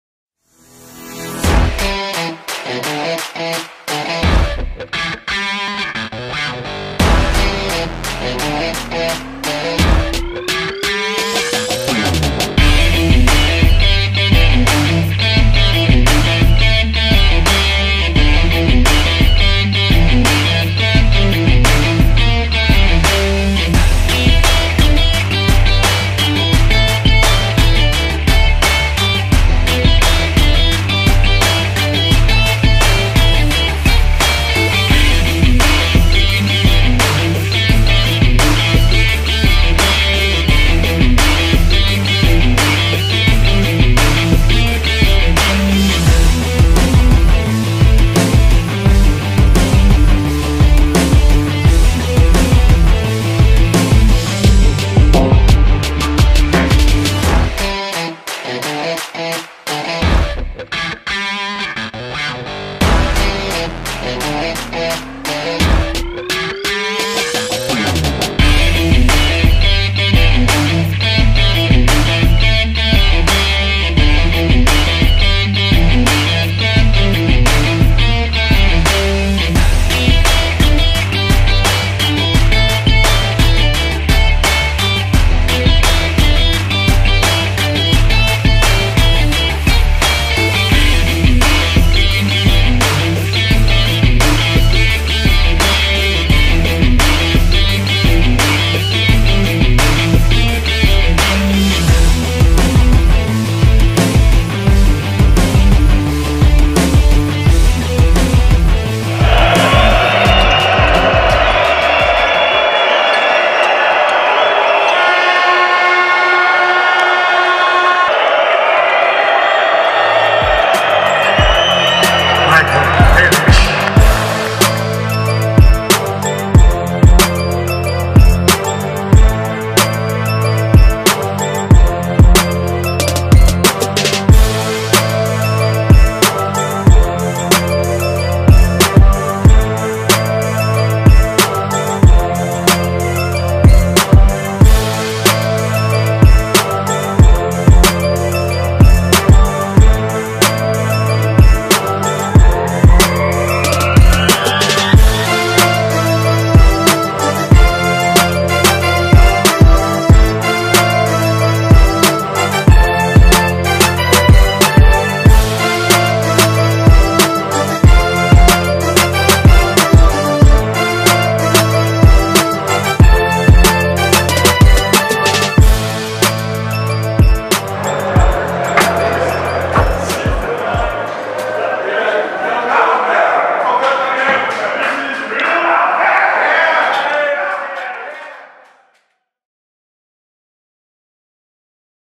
Galatasaray Doğa Sigorta - Darüşşafaka Tekfen | Maç Hikayesi
Tahincioğlu Basketbol Süper Ligi 24. hafta mücadelesinde Galatasaray Doğa Sigorta'yı deplasmanda 80-64 mağlup ettiğimiz müsabakanın öncesi, maç anı ve maç sonrası görüntülerinden oluşturduğumuz video sizlerle.